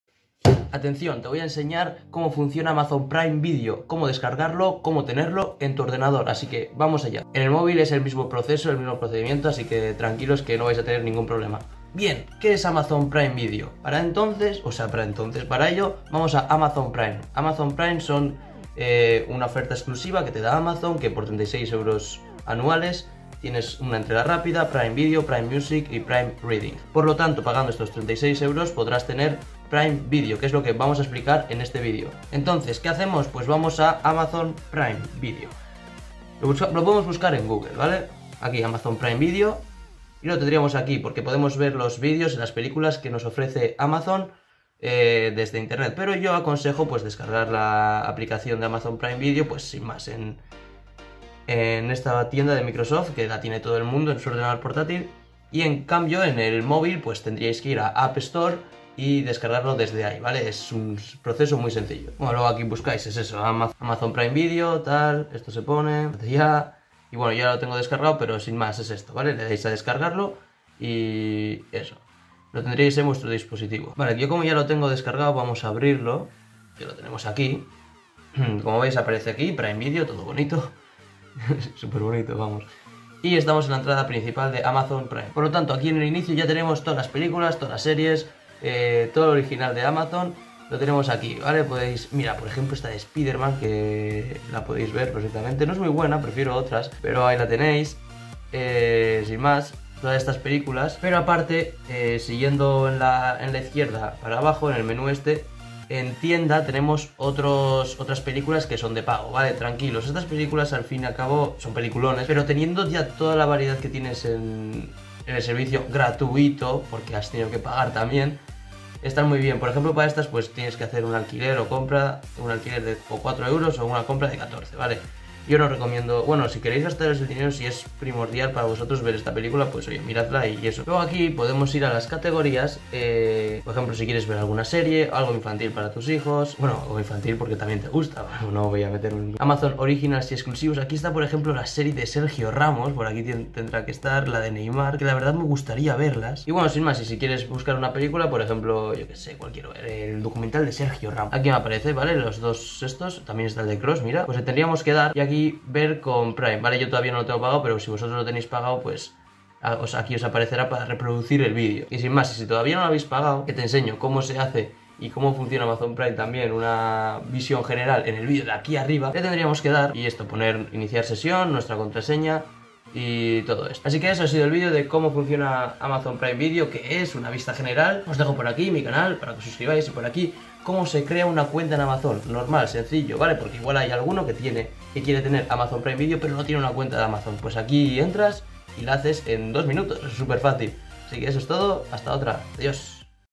Como FUNCIONA AMAZON PRIME VIDEO 2022 ✅ (TE EXPLICO TODO) #8
⚡AQUI TIENES UN BREVE RESUMEN DE LAS CARACTERISTICAS DE AMAZON PRIME VIDEO, ¡NO ESPERES A VERLO!⚡

Espero que os haya gustado el video, en caso de tener alguna duda ponerla en los comentarios e intentaré responderos.